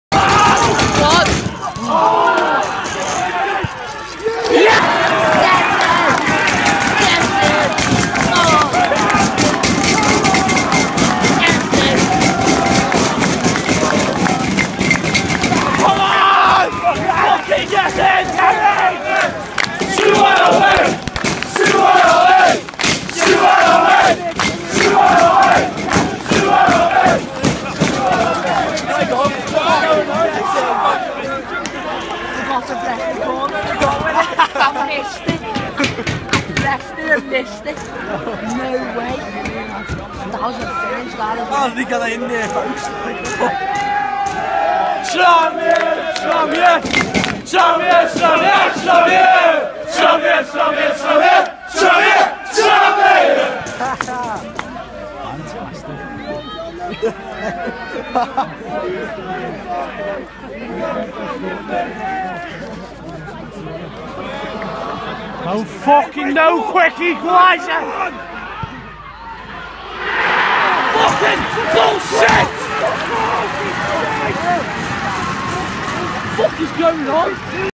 Tranmere Fans At Guiseley 08/03/16
541 Tranmere Fans Made The 2 in a Half Hour Journey To Guiseley On A Tuesday Night, For A Poor Performance & A 2-2 Draw! Was A Very Bad Game For Us! This Is Just As We Scored Our Second Goal, But At The End Of The Video, They Literally Equalise Straight Away! Long Away Trip To Dover On Saturday!